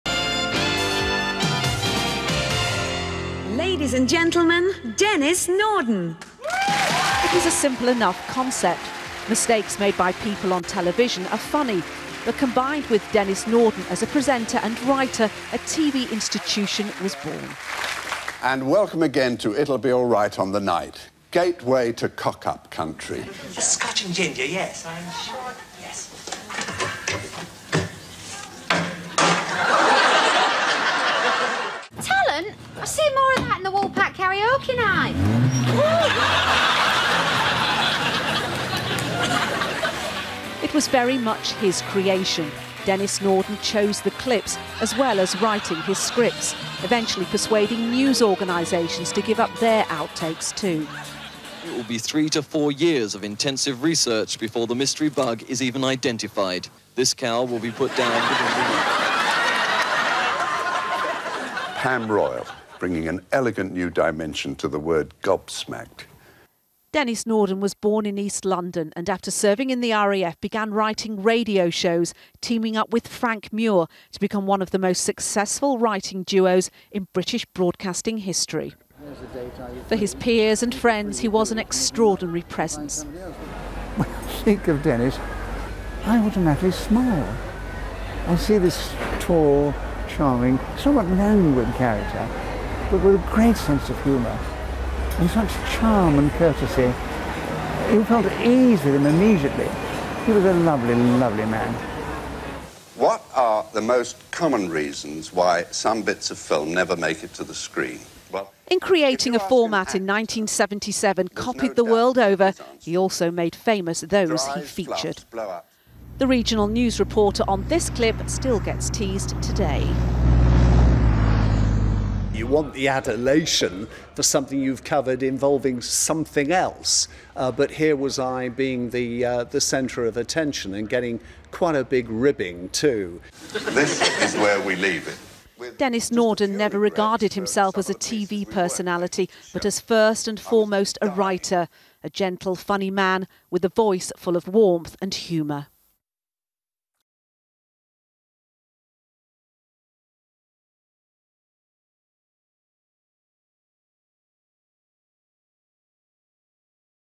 'It'll Be Alright On The Night' host Denis Norden dies aged 96 | ITV News
TV presenter and comedy writer Denis Norden has died aged 96, his family said.

The "It'll Be Alright On The Night" host died on Wednesday morning after spending “many weeks” at the Royal Free Hospital in north London.

Norden presented the ITV bloopers show for 29 years until his retirement in 2006.

A family statement said: "We’d like to say a huge thank you to all the dedicated staff and doctors who have looked after him – with much devotion.

"A wonderful dad, a loving grandfather and great great-grandfather – he gave his laughter-mongering to so many.

"He will be in our hearts forever."